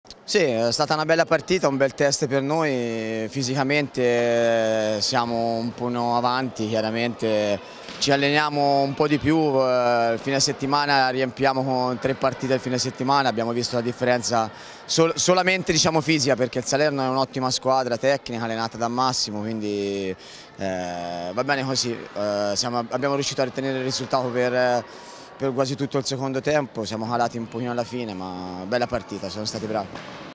Coppa Italia Serie A2 - G.8 - Intervista a Franco Polverini (Allenatore Follonica Hockey)
hockeypista hpseriea2 - Coppa Italia Serie A2 - Qualificazione - Girone D - Anticipo - Giornata n.8 - Campolongo Hospital Salerno x Follonica H. - PalaTumilieri di Salerno - Sabato 11 novembre 2023 ore 20:00